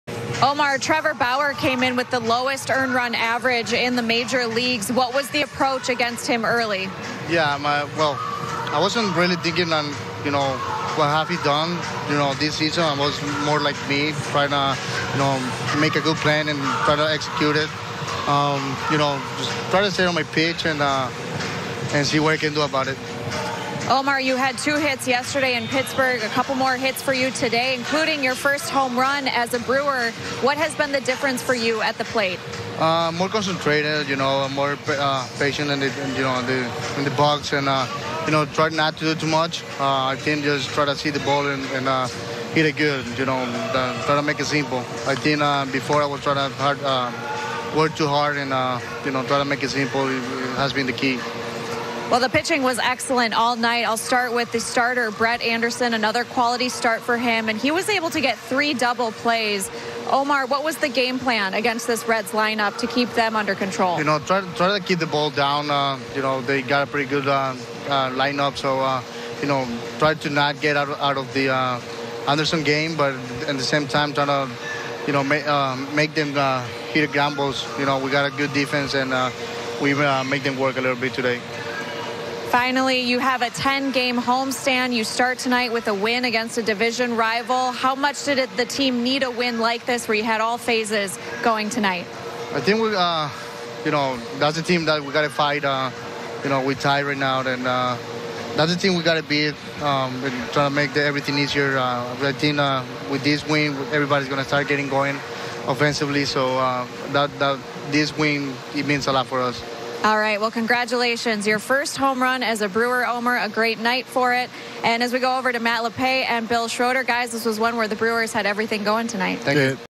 Brewers catcher Omar Narvaez keeping things simple at the plate
Omar Narvaez went 2-for-4 with a homer during the Milwaukee Brewers' 4-2 win over Cincinnati.